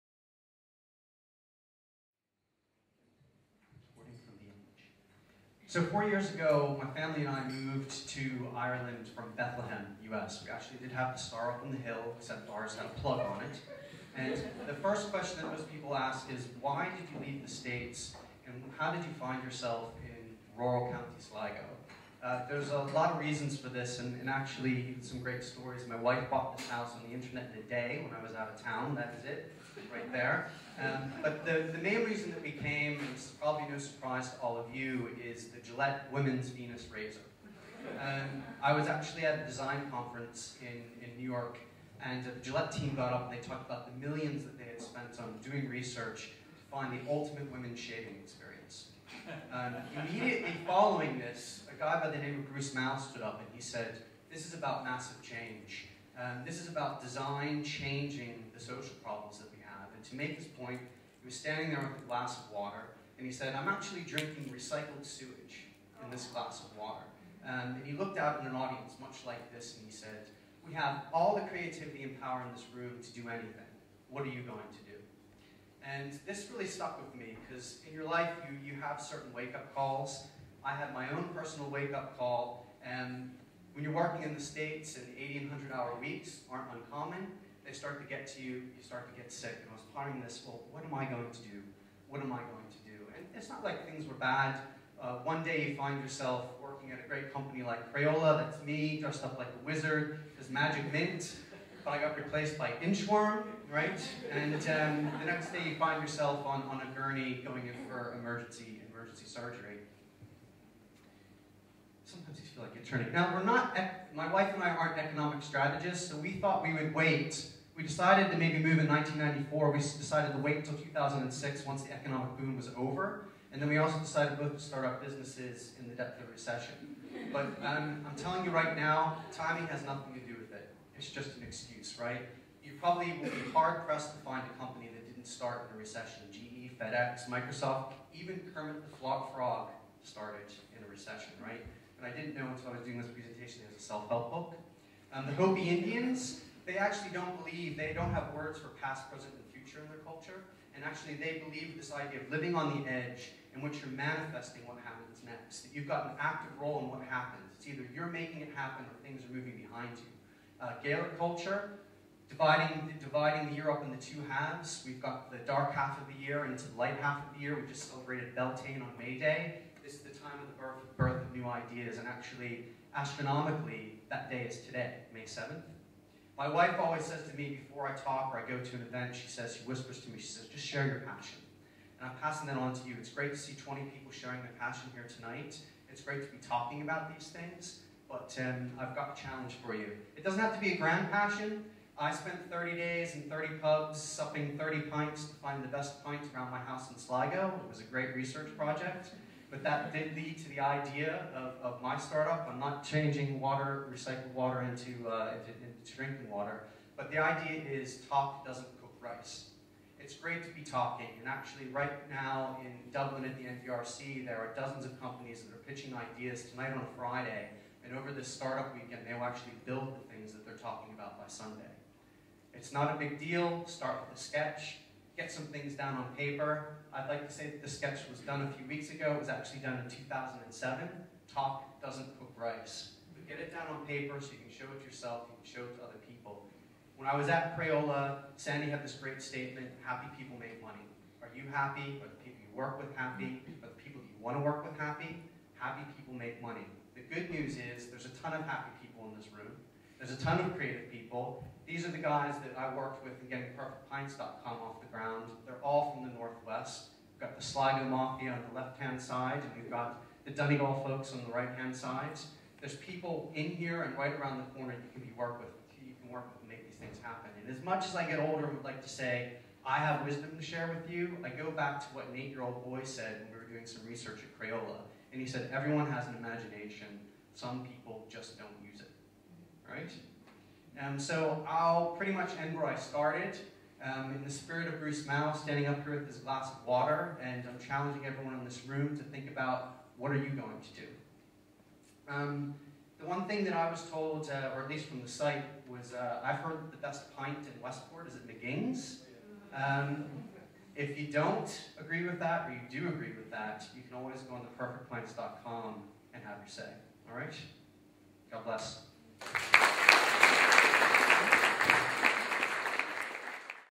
Justin Knecht
Entrepreneur Justin Knecht, an imported "design evangelist" bottled in Sligo, traces his hitherto unsuspected links between good design and the the perfect pint: you can see the results, and offer your take, at perfectpints.com